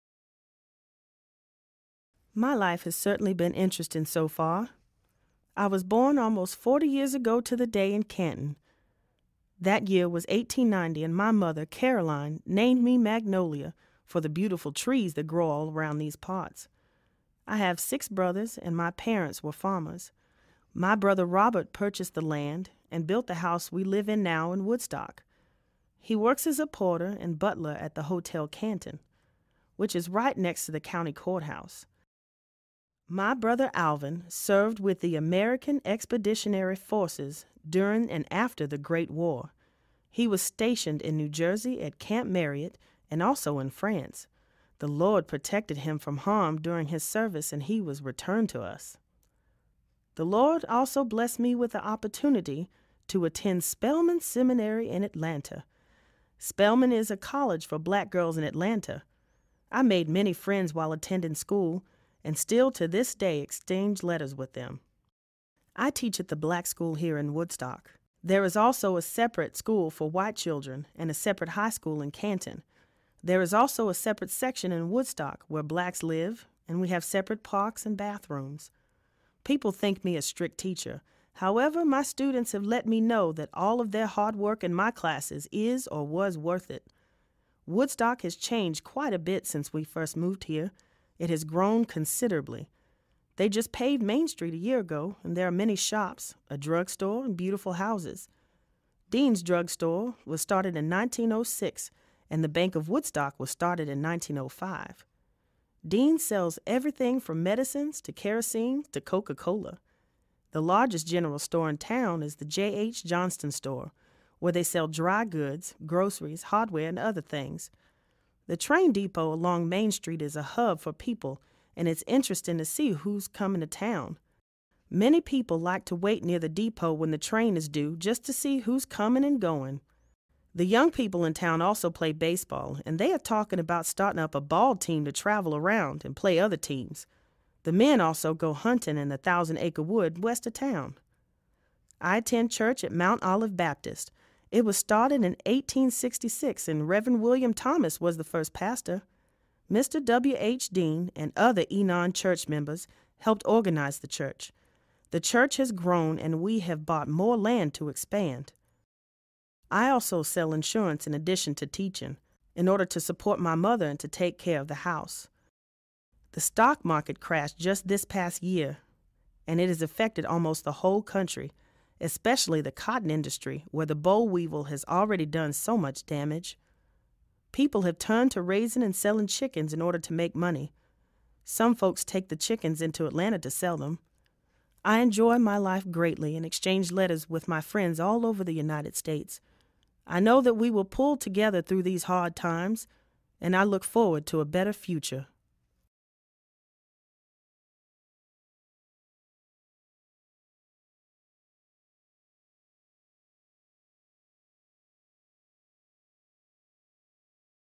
Magnolia Thomas
She was a graduate of Spelman Seminary in Atlanta and she taught school in Woodstock.